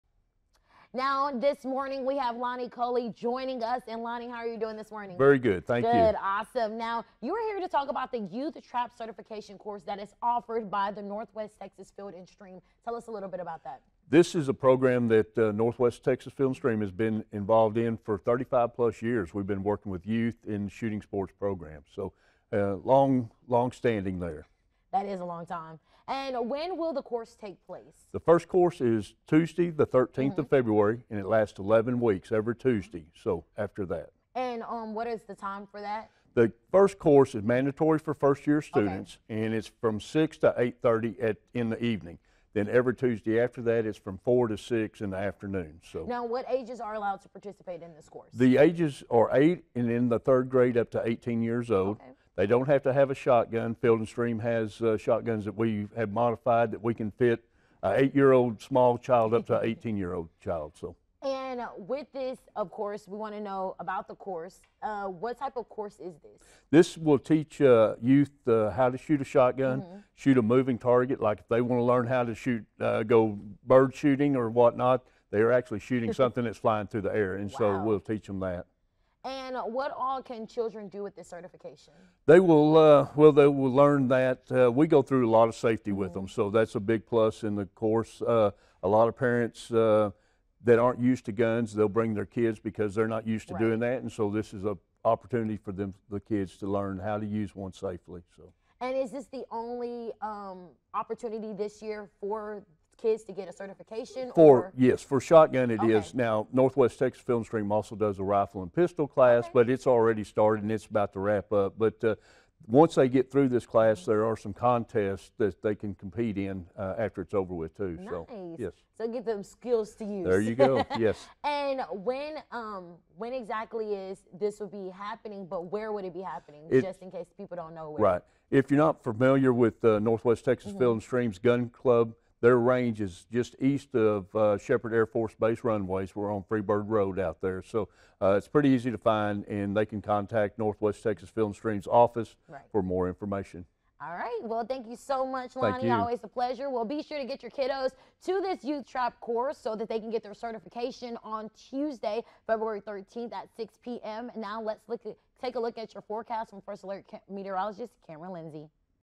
Youth Trap Certification Course
Lonnie Culley joined us in the studio this morning to talk about an upcoming course for all youth in the area wanting to learn about shotgun shooting.